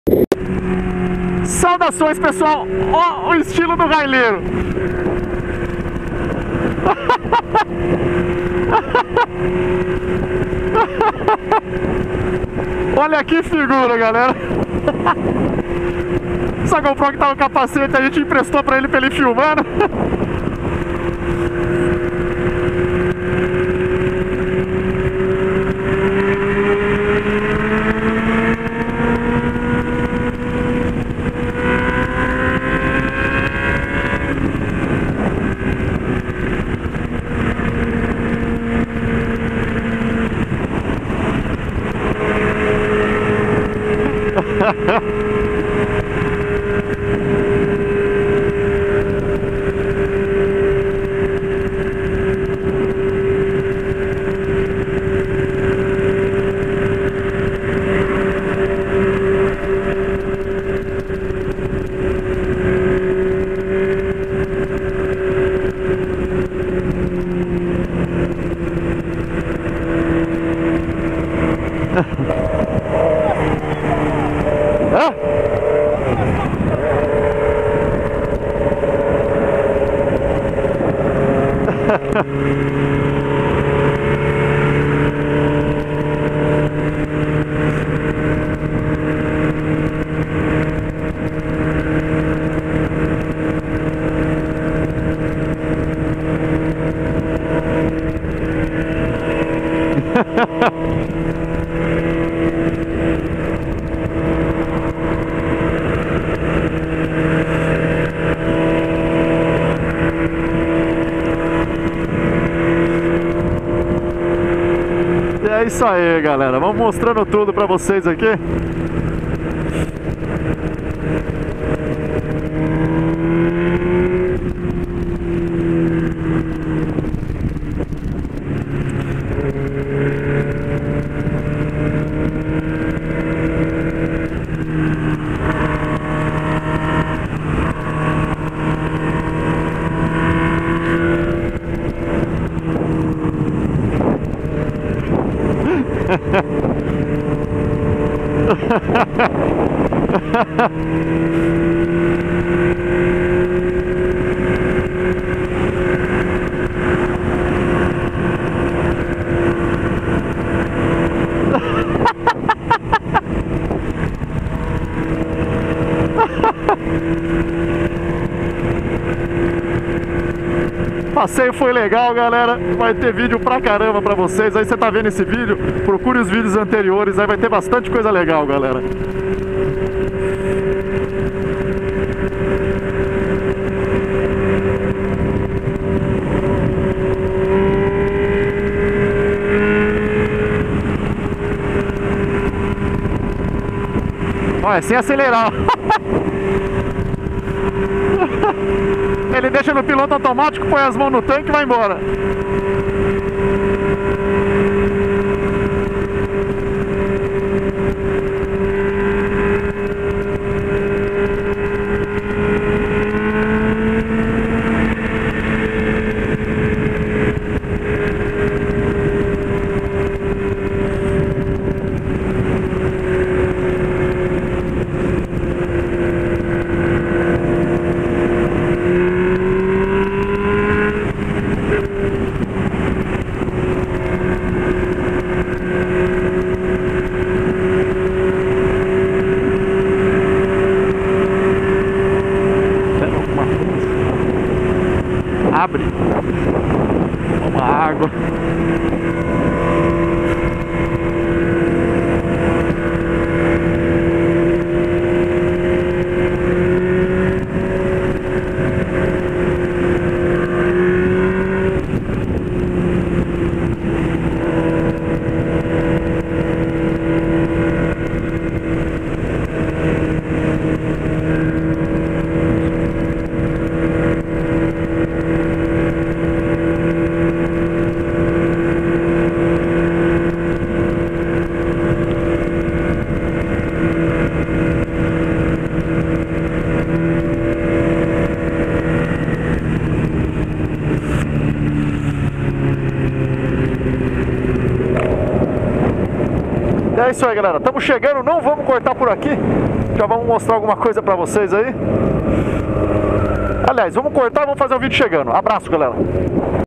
HARLEY CAUSANDO NO ROLÊ, kkkkkk - HONDA HORNET BRANCA ABS - MUMU
Rua General Osório, 511 - Centro São Paulo - SP

JESKAP - FULL GP ALUMINIO CARBON -

Filmado com GOPRO HERO2